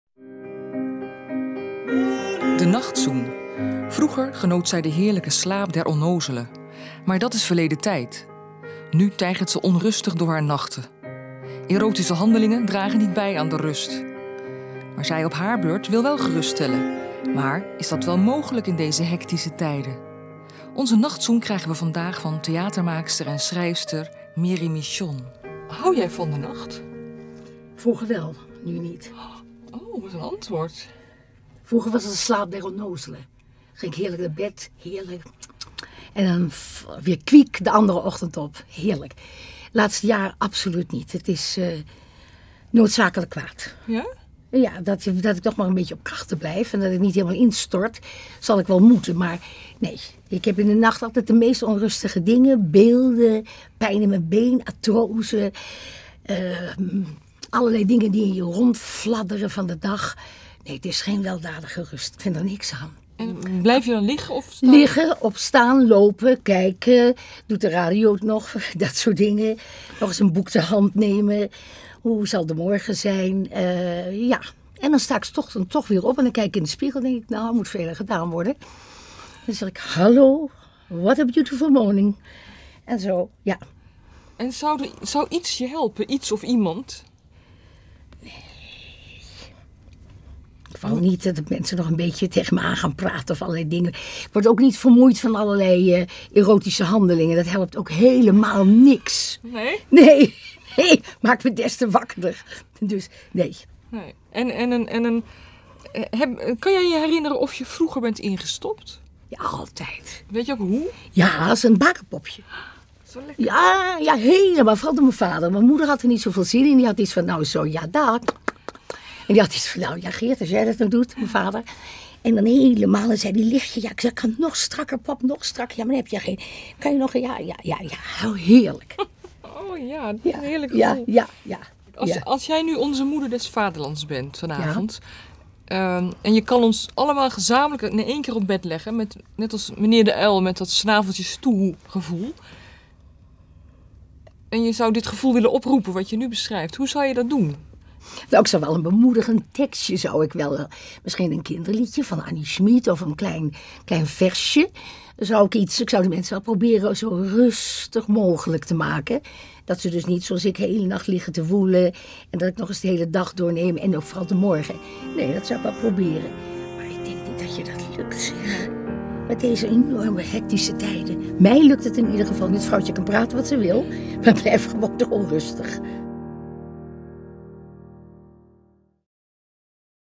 Mary Michon
Vroeger sliep zij de heerlijke slaap der onnozelen. Maar dat is verleden tijd. Nu tijgert ze onrustig door haar nachten. Erotische handelingen dragen niet bij aan haar rust. 1haar beurt wel geruststellen, maar is dat niet onmogelijk in deze hektische tijden? Onze nachtzoen krijgen we van theatermaakster en schrijfster Mary Michon.